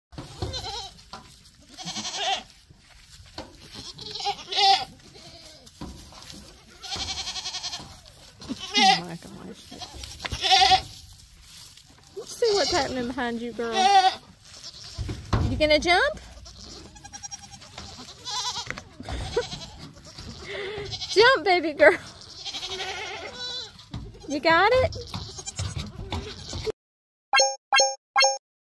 Billy Goats Steal the Show
While I was recording the newest babies, the daddies stole the show.